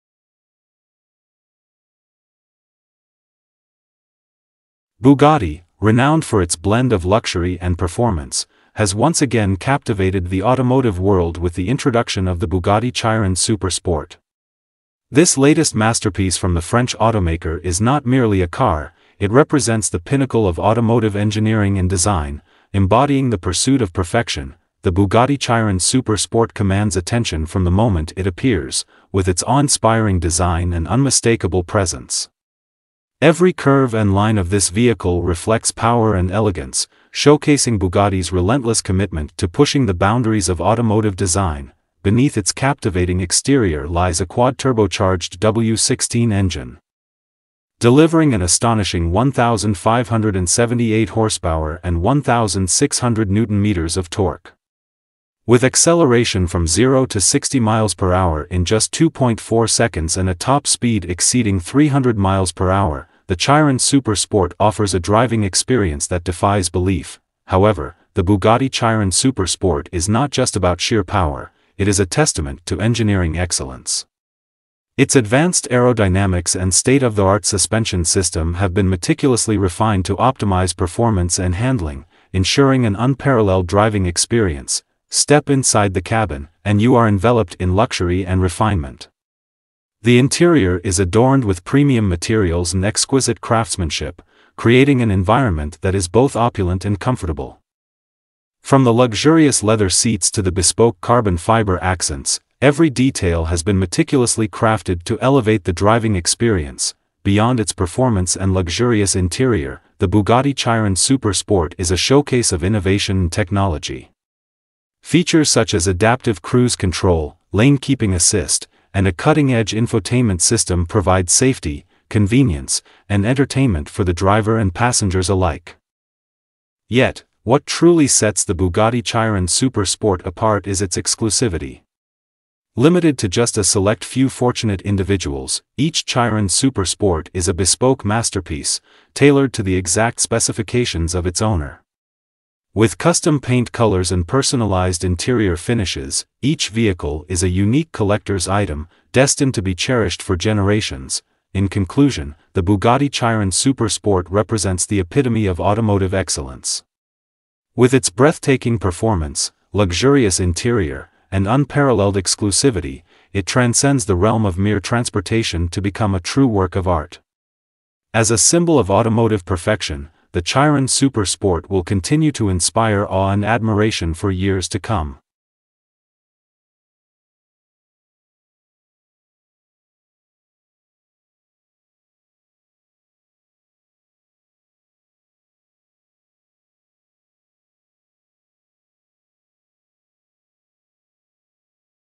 Unveiling the Bugatti Chiron Super Sport: The Epitome of Automotive Excellence
Bugatti, renowned for its blend of luxury and performance, has once again captivated the automotive world with the introduction of the Bugatti Chiron Super Sport. This latest masterpiece from the French automaker is not merely a car; it represents the pinnacle of automotive engineering and design, embodying the pursuit of perfection.

The Bugatti Chiron Super Sport commands attention from the moment it appears, with its awe-inspiring design and unmistakable presence. Every curve and line of this vehicle reflects power and elegance, showcasing Bugatti's relentless commitment to pushing the boundaries of automotive design.

Beneath its captivating exterior lies a quad-turbocharged W16 engine, delivering an astonishing 1,578 horsepower and 1,600 Nm of torque. With acceleration from 0 to 60 mph in just 2.4 seconds and a top speed exceeding 300 mph, the Chiron Super Sport offers a driving experience that defies belief.

However, the Bugatti Chiron Super Sport is not just about sheer power; it is a testament to engineering excellence. Its advanced aerodynamics and state-of-the-art suspension system have been meticulously refined to optimize performance and handling, ensuring an unparalleled driving experience.

Step inside the cabin, and you are enveloped in luxury and refinement. The interior is adorned with premium materials and exquisite craftsmanship, creating an environment that is both opulent and comfortable. From the luxurious leather seats to the bespoke carbon fiber accents, every detail has been meticulously crafted to elevate the driving experience.

Beyond its performance and luxurious interior, the Bugatti Chiron Super Sport is a showcase of innovation and technology. Features such as adaptive cruise control, lane-keeping assist, and a cutting-edge infotainment system provide safety, convenience, and entertainment for the driver and passengers alike.

Yet, what truly sets the Bugatti Chiron Super Sport apart is its exclusivity. Limited to just a select few fortunate individuals, each Chiron Super Sport is a bespoke masterpiece, tailored to the exact specifications of its owner. With custom paint colors and personalized interior finishes, each vehicle is a unique collector's item, destined to be cherished for generations.

In conclusion, the Bugatti Chiron Super Sport represents the epitome of automotive excellence. With its breathtaking performance, luxurious interior, and unparalleled exclusivity, it transcends the realm of mere transportation to become a true work of art. As a symbol of automotive perfection, the Chiron Super Sport will continue to inspire awe and admiration for years to come.

Bugatti Chiron Super Sport: A Dream Unleashed